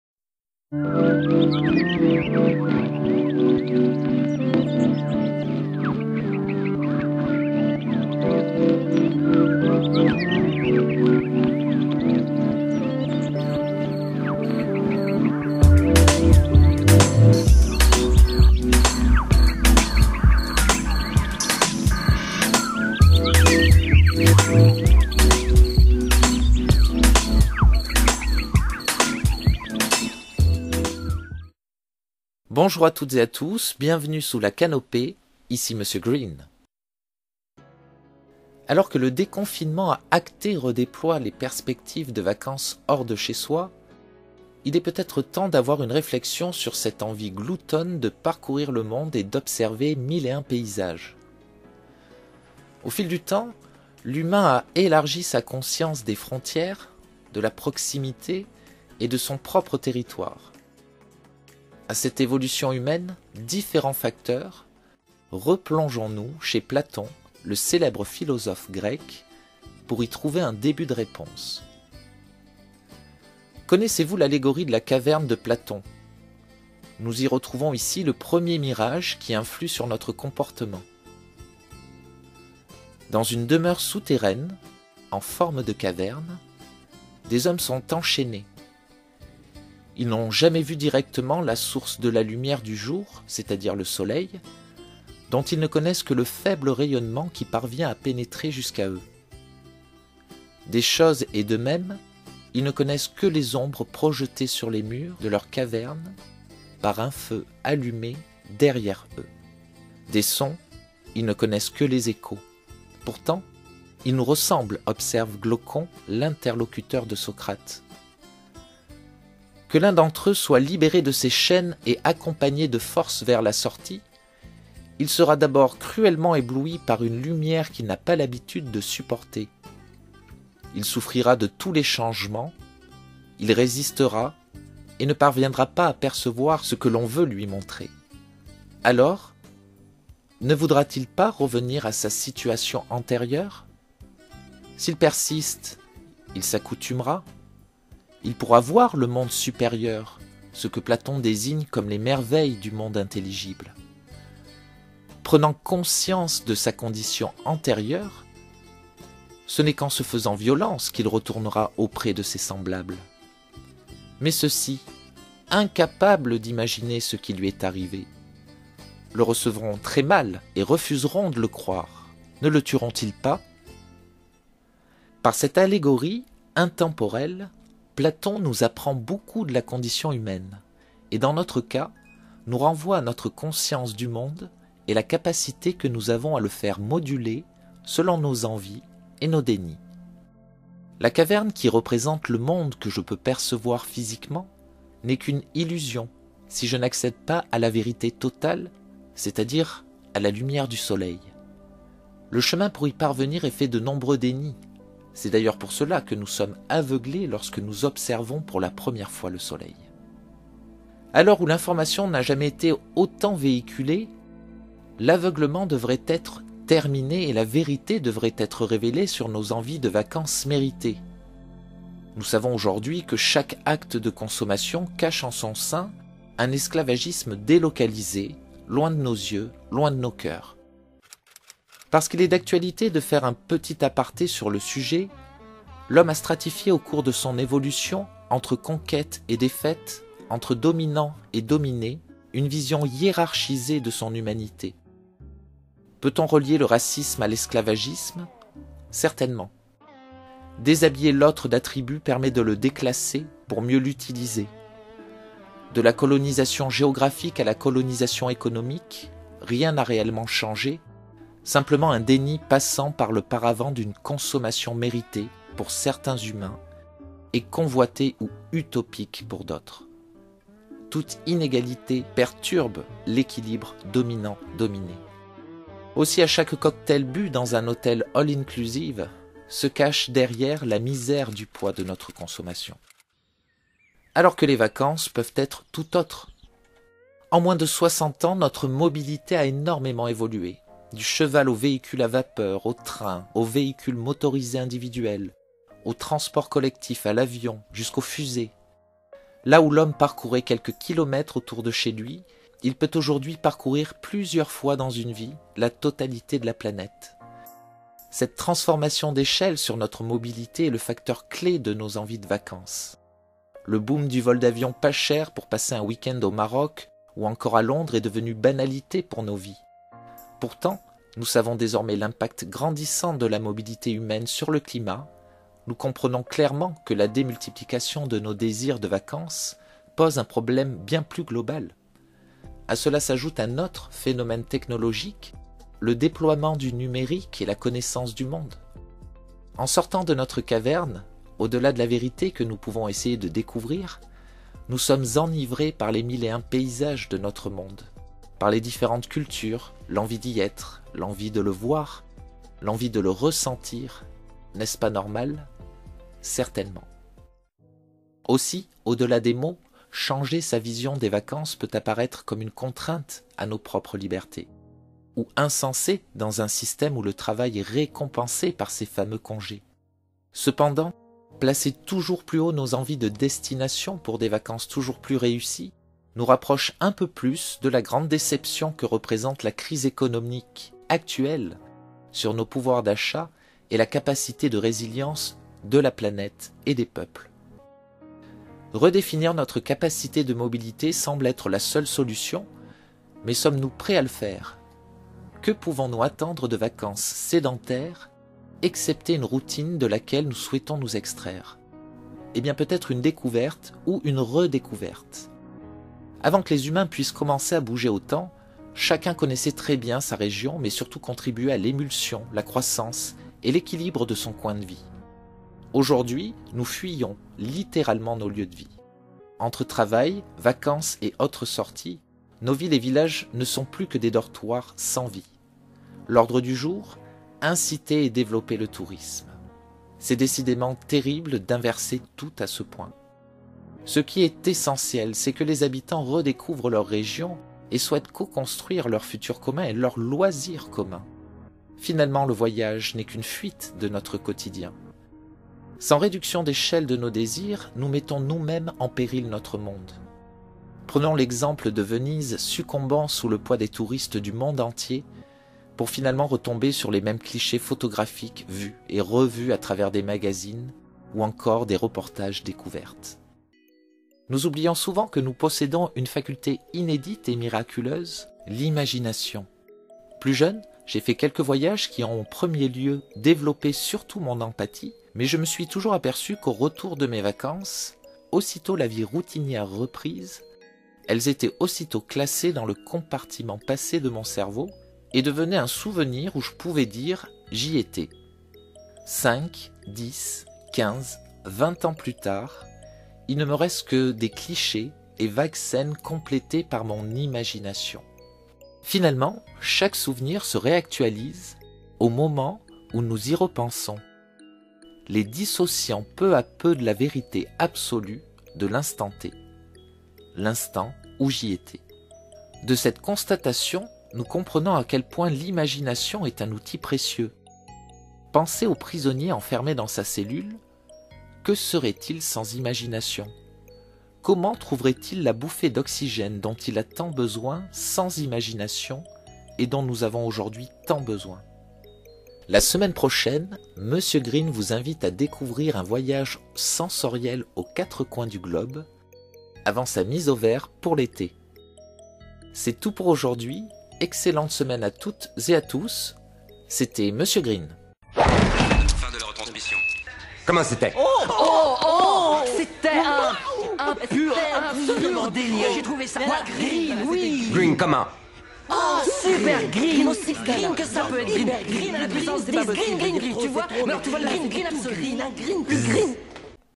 Eté 2020 : vacances lointaines ou loisirs locaux !
Alors que le déconfinement est acté pour relancer l'économie et le tourisme, comment envisagez-vous vos vacances d'été ? les vulnérabilités mises à jour par le coronavirus, ont-elles également modifié vos envies de vacances ?